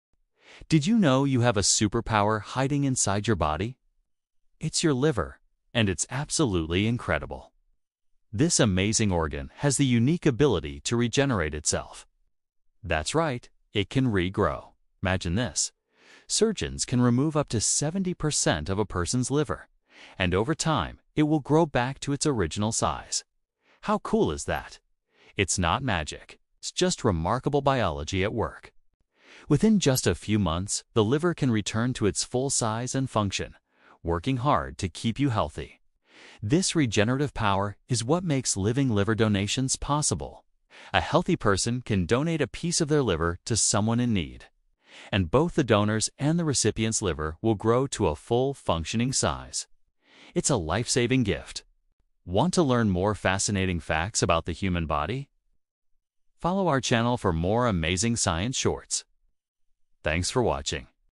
Your Liver Can Regrow Itself
The liver can regrow itself even after major damage.